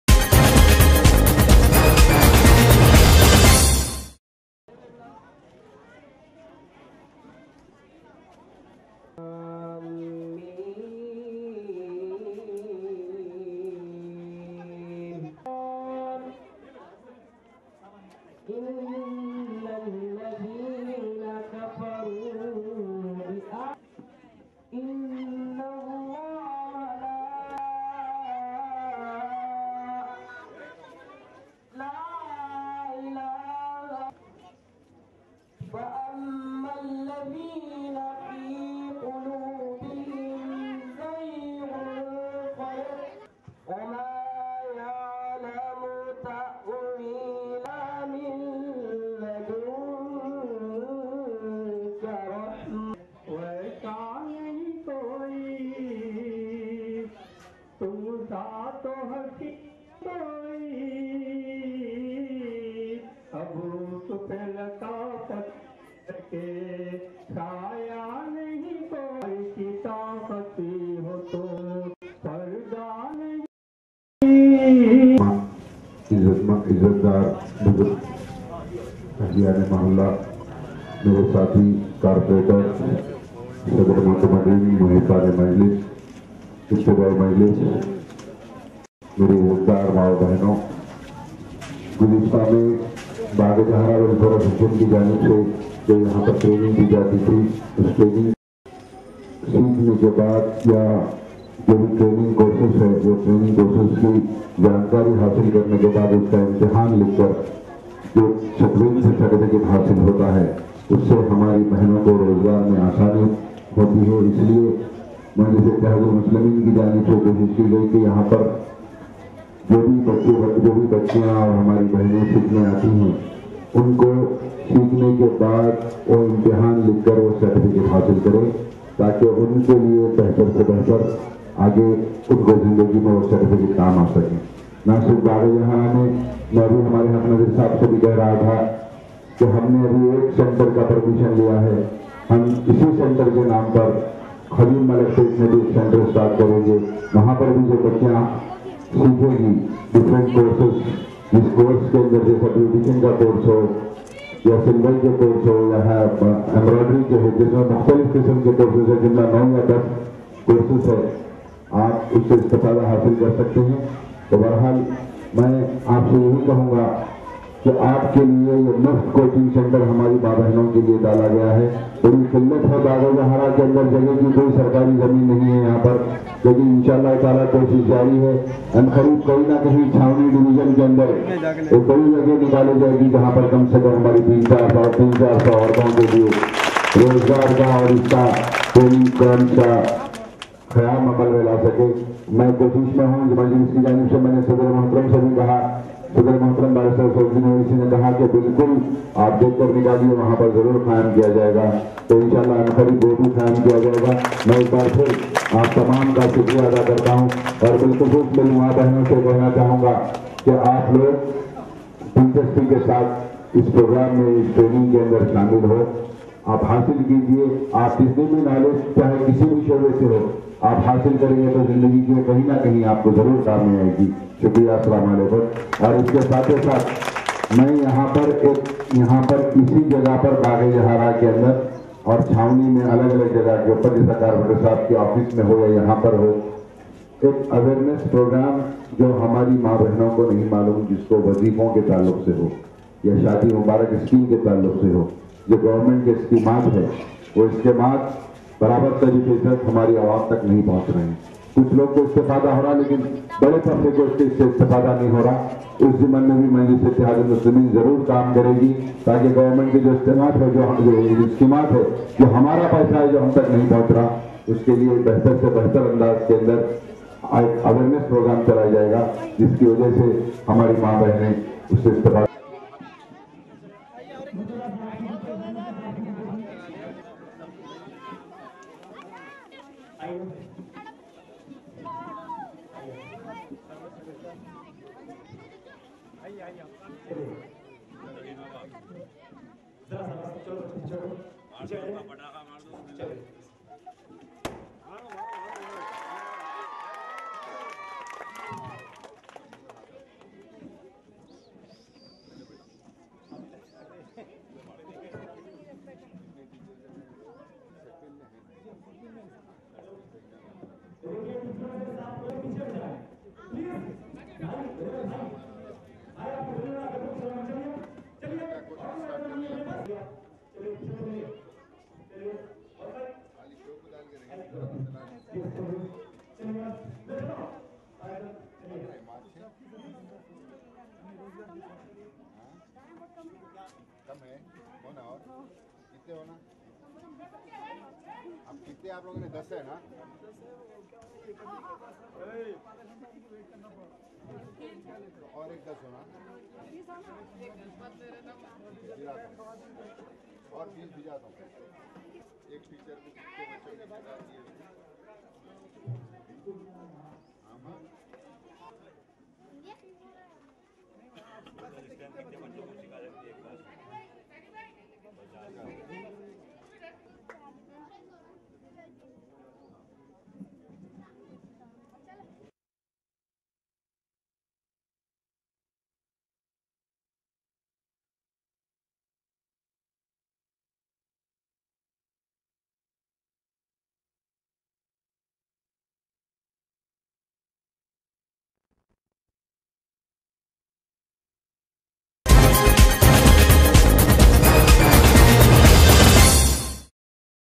Free summer camp at Bagh E Jahan Aara inaugurated by MIM MLA Ahmed Balala
Free summer camp at Bagh E Jahan Aara inaugurated by MIM MLA Ahmed Balala. Author: Rayees Azam